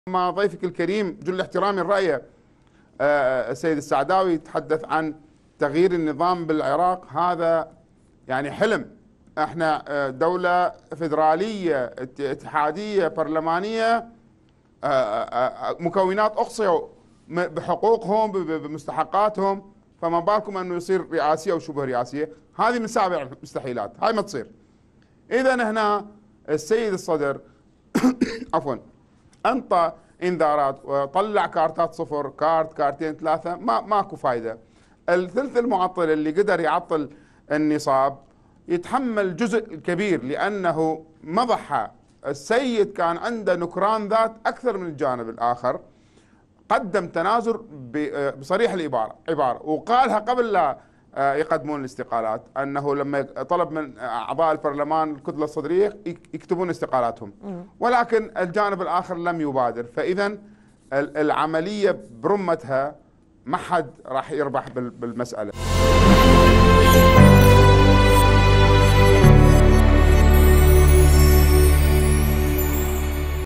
باجلان: نحن بلد فيدرالي برلماني والتحول الى النظام الرئاسي من سابع المستحيلات
10891📺 افقي
10892📺 افقي
10893📺 افقي